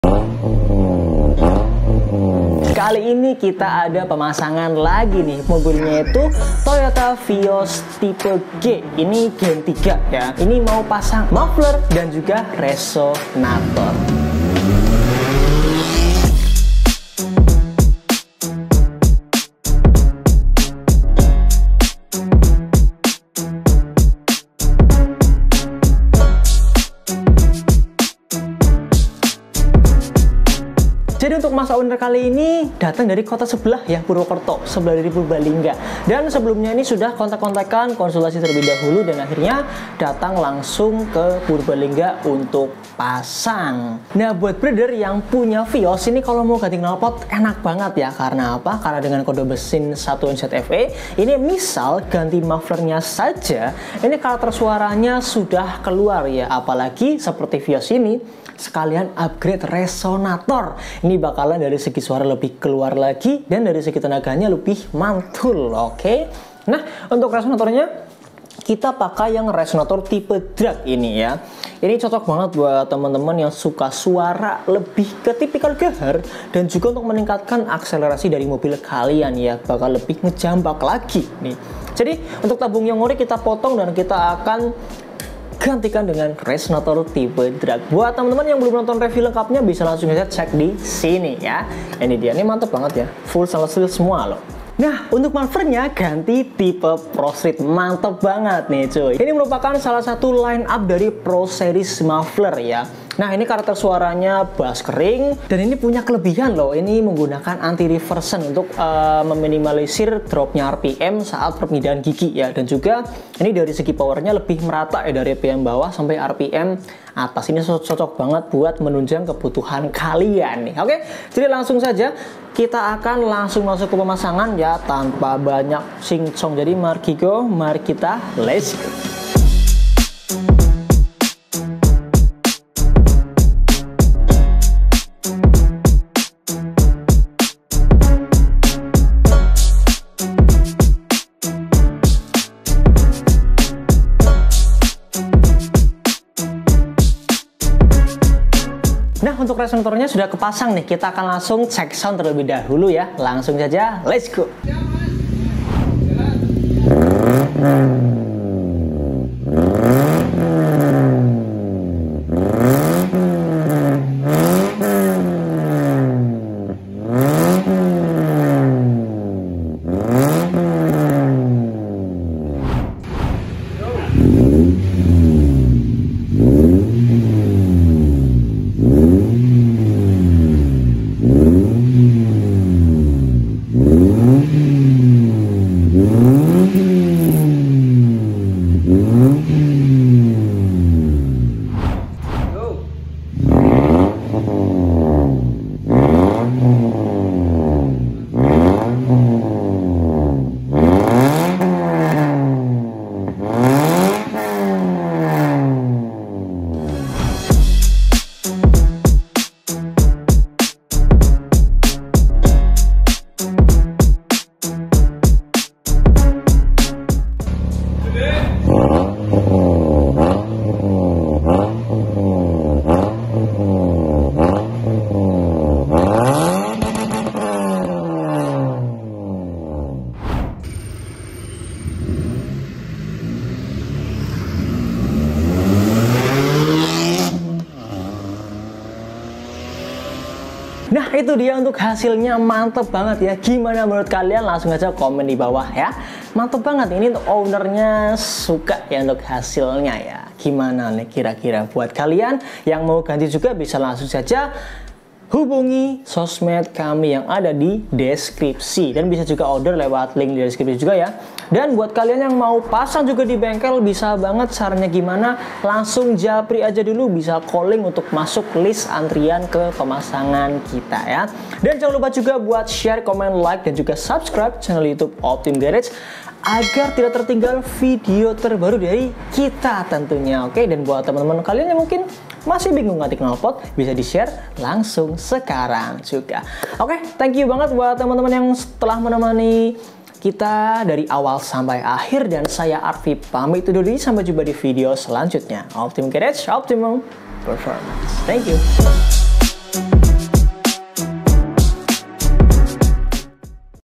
VIOS UPGRADE SUARA DAN PERFORMA!!! | MUFFLER PRO STREET & RESONATOR DRAG S NDK Exhaust
Toyota Vios Ganti Muffler Pro Street Dan Resonator Drag S

Spesifikasi :
- Muffler Pro Street
- Resonator Drag S
- Header Ori